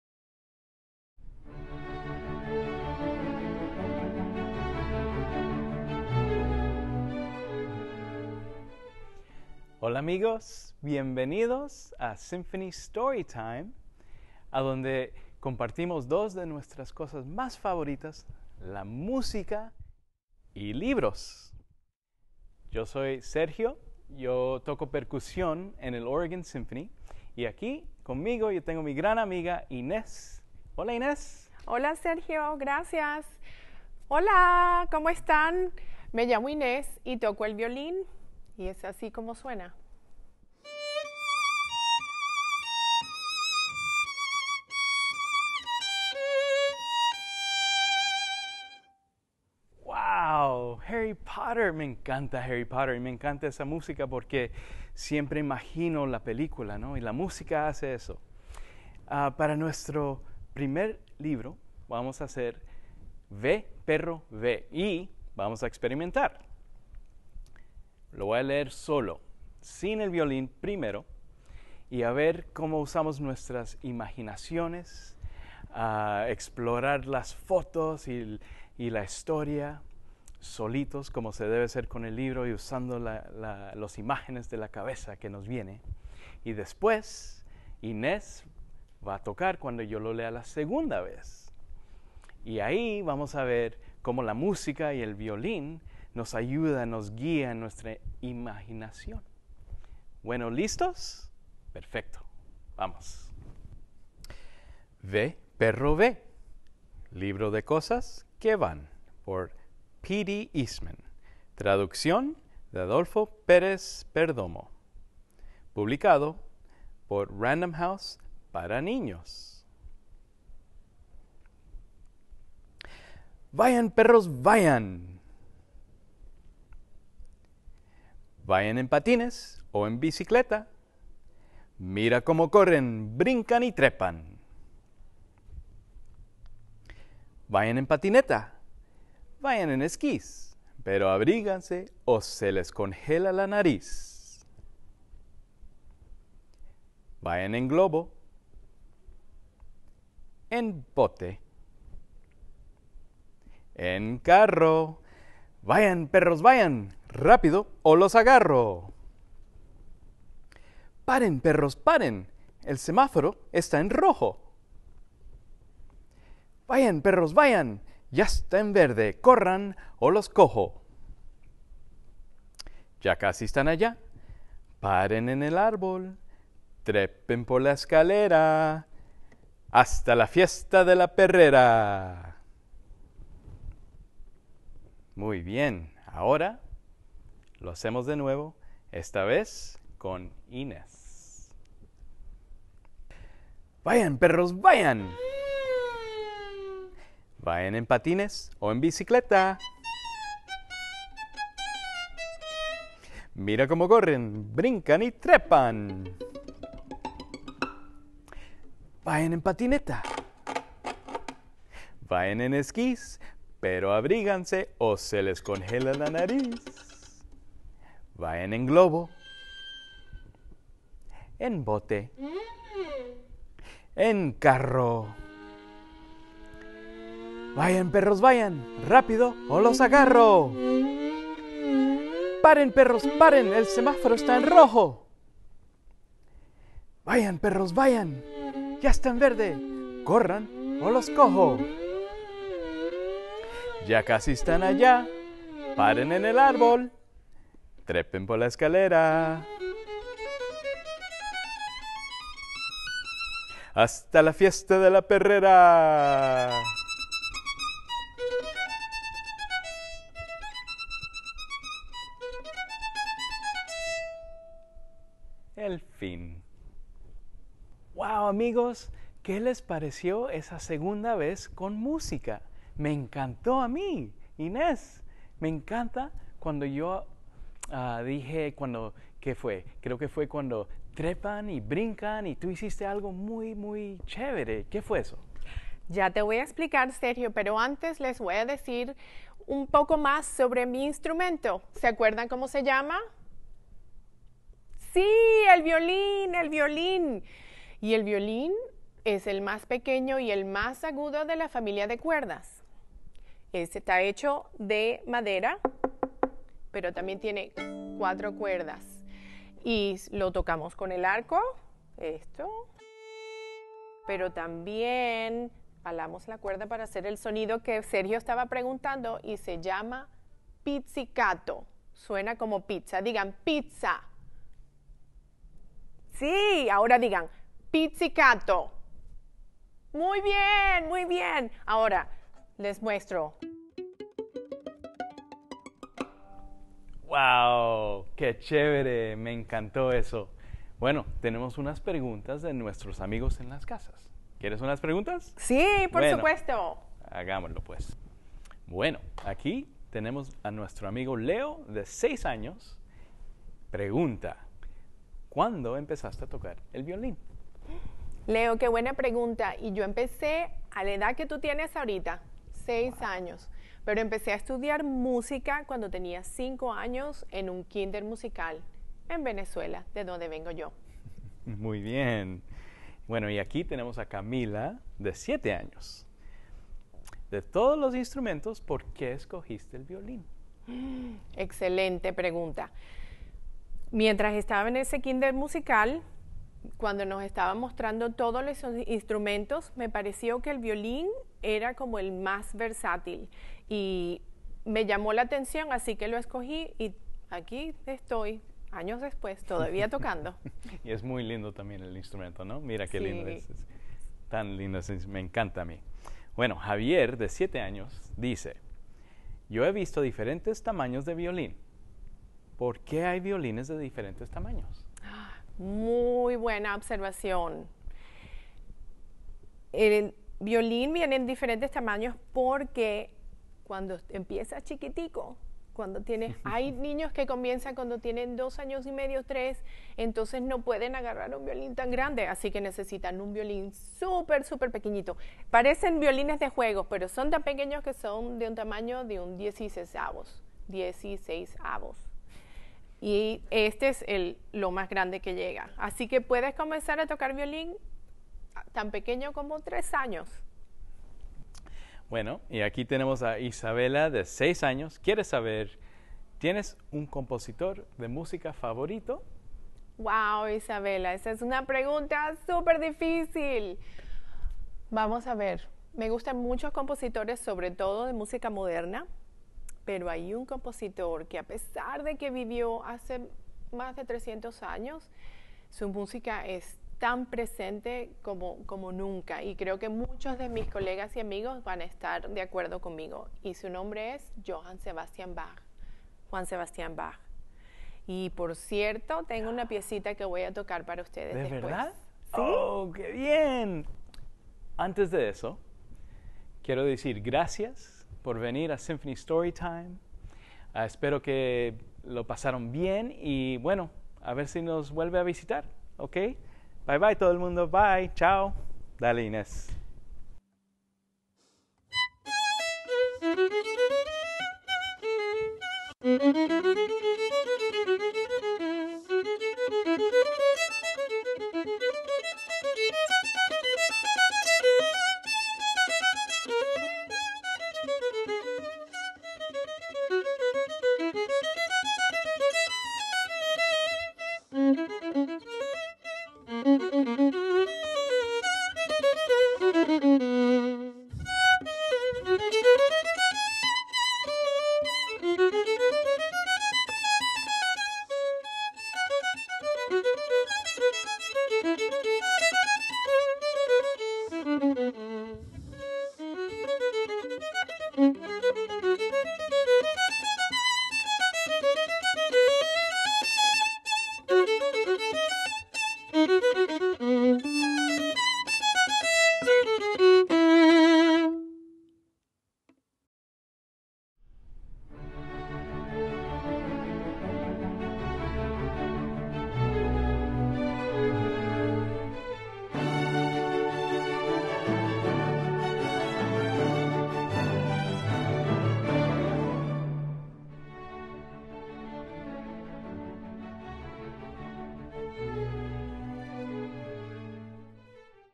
Symphony Storytime • “Ve, Perro. ¡Ve!” presentando el violín
Motley mutts viaja en bicicleta, triciclo, patines, esquís, básicamente en lo que sea, para poder ir a la fiesta que se celebra en el árbol

Escrito por P.D. Eastman y traducido por Adolfo Pérez.
Anfitrión Sergio Carreno y presentación musical por Inés Voglar Belgique con su violín.

Mira mas videos ahora en la pagina web